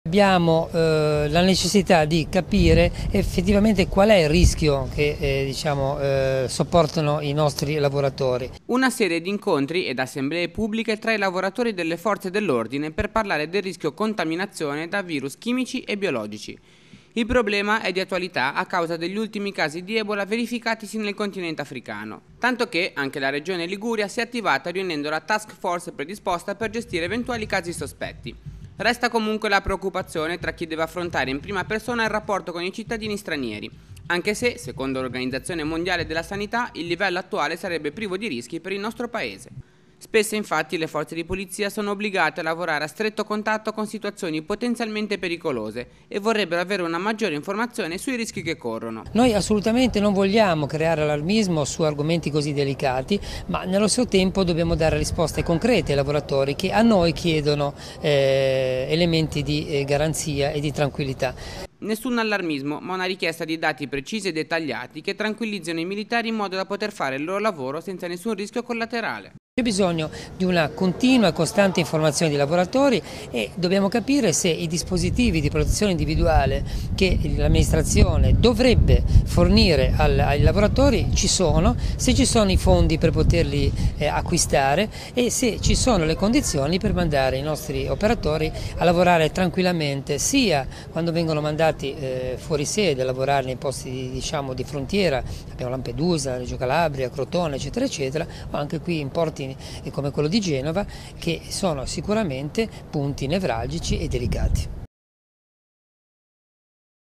Rischio Ebola, la polizia: "Nessun allarmismo, ma rassicurazioni"
Una serie di incontri ed assemblee pubbliche tra i lavoratori delle forze dell'ordine per parlare del rischio contaminazione da virus chimici e biologici. Il problema è di attualità, a causa degli ultimi casi di Ebola verificatisi nel continente africano